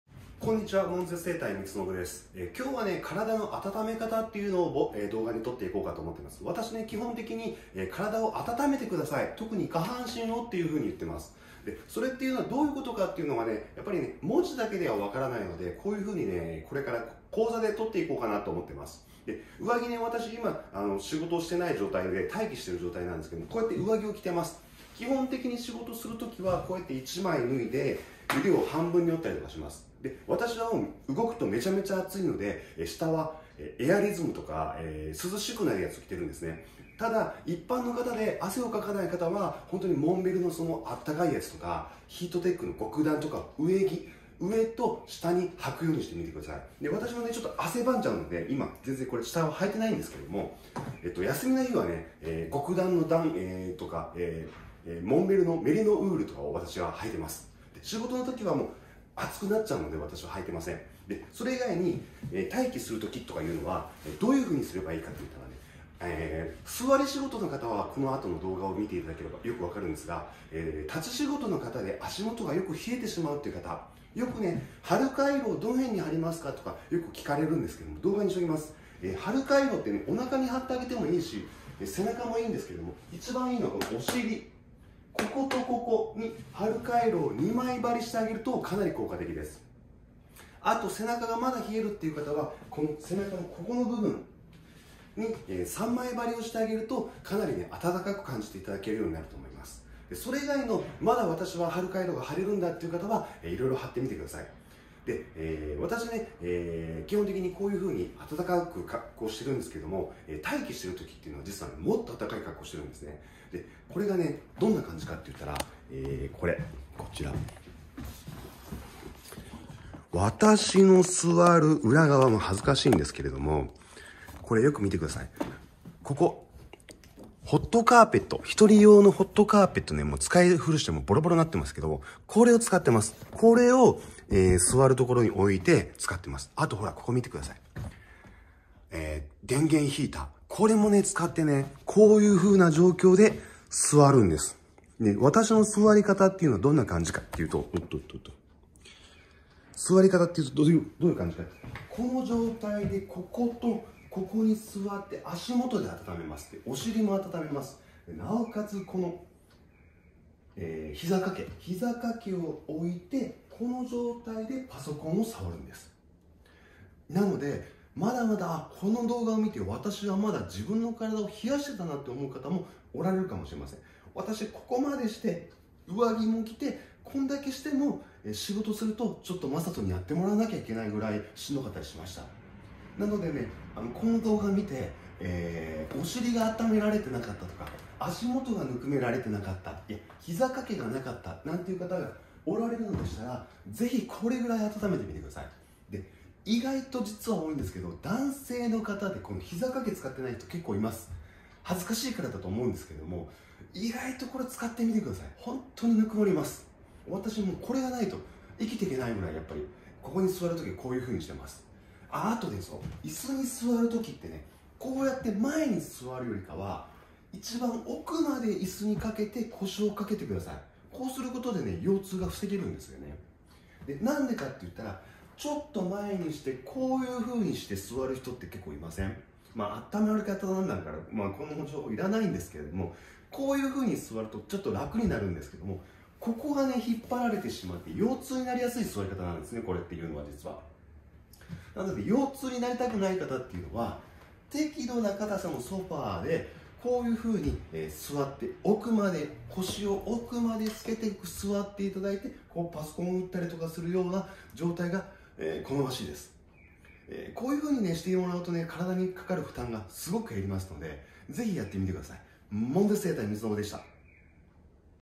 悶絶整体。万病を防ぐ身体の温め方。肩こり腰痛を寒い冬には温めることで痛みやこりを解消できる。セルフメンテナンスで腰痛を回復extreme shiatu
肩こりや、腰痛は冷えから出てきます
普段の生活をこれだけ温めていただければ
腰痛や肩こりを防ぐことができます

ぜひ誰でもできるのでやってみてください。

悶絶整体みつのぶは奈良県にあります。
近鉄『富雄駅』下車１分。
みつのぶカイロプラクティック詳しくは下記リンクよりどうぞ

新着順

他のYouTubeのオススメはこちら